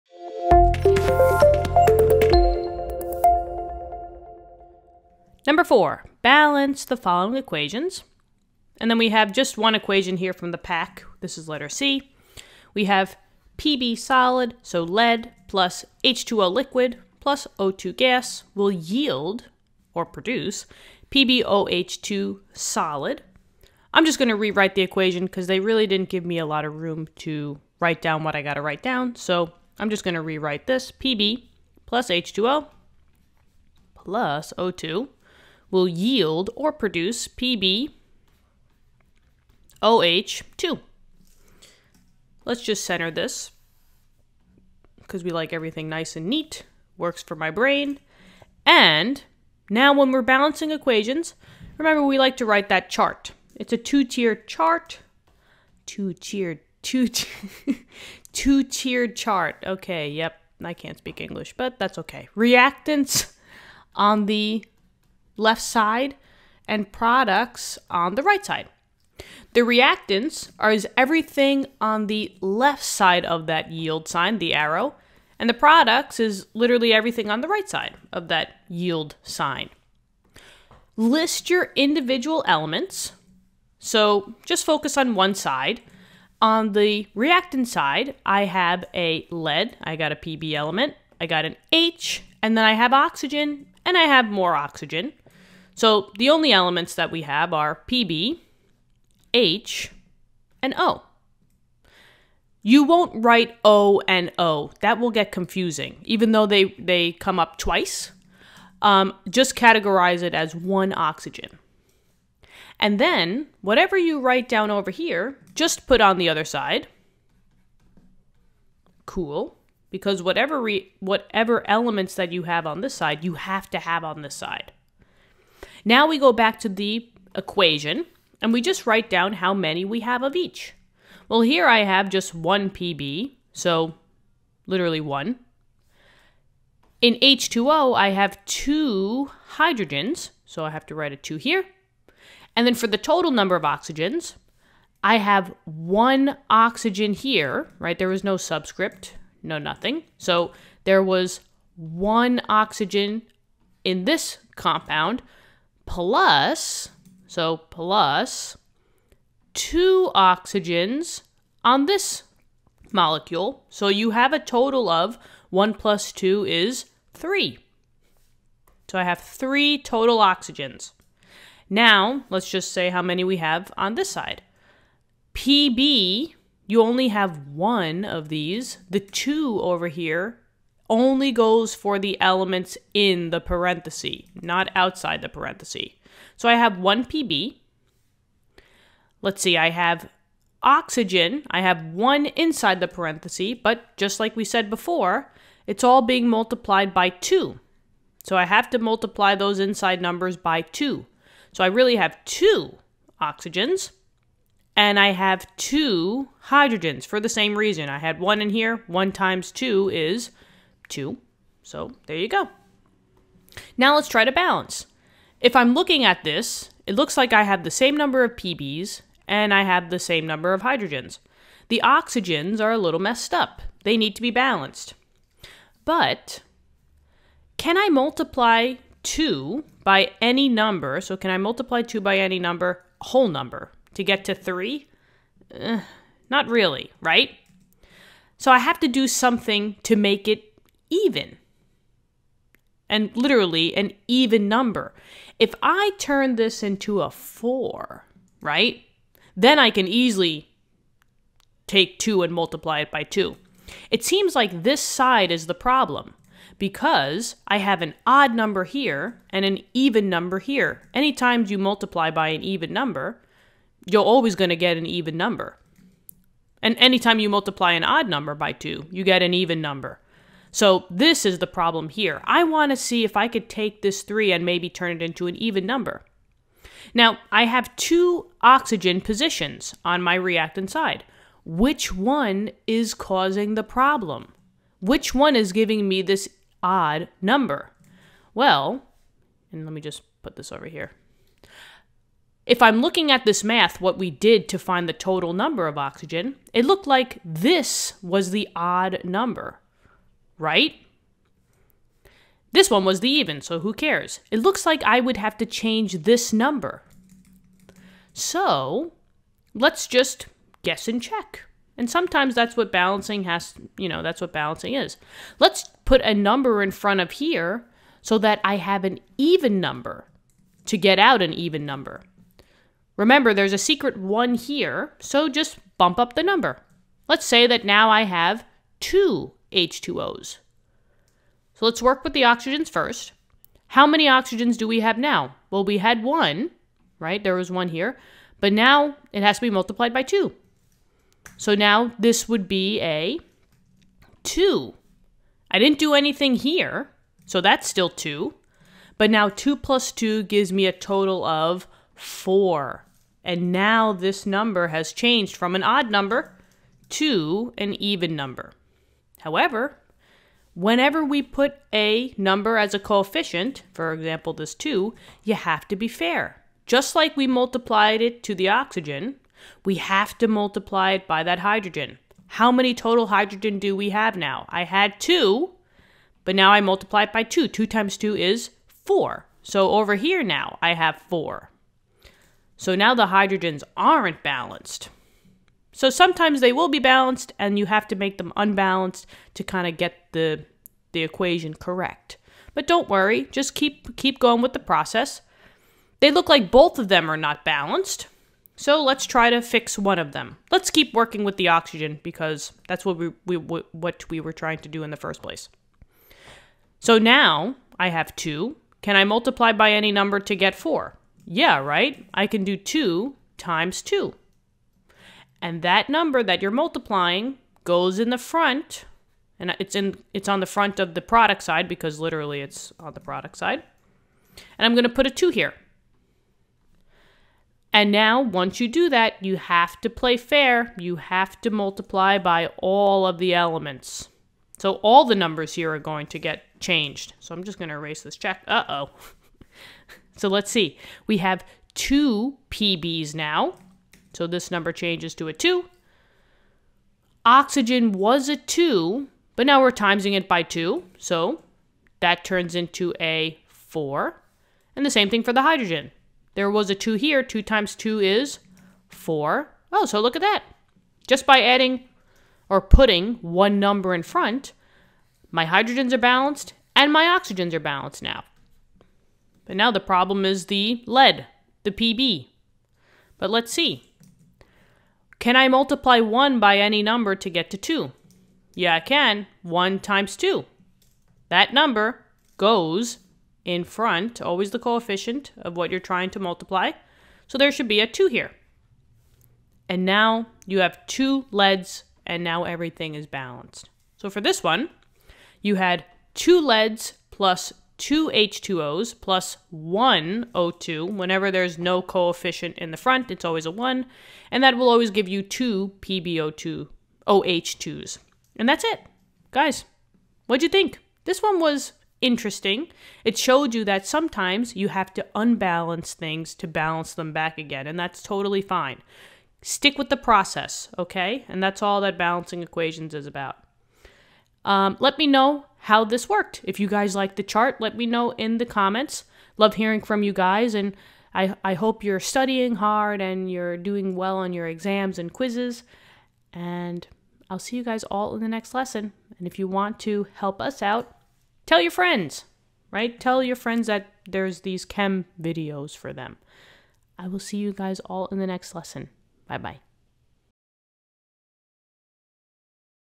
4.4c | Balance: Pb(s) + H2O(l) + O2(g) → Pb(OH)2(s)
Balance the following equations:
Pb(s) + H2O(l) + O2(g) → Pb(OH)2(s)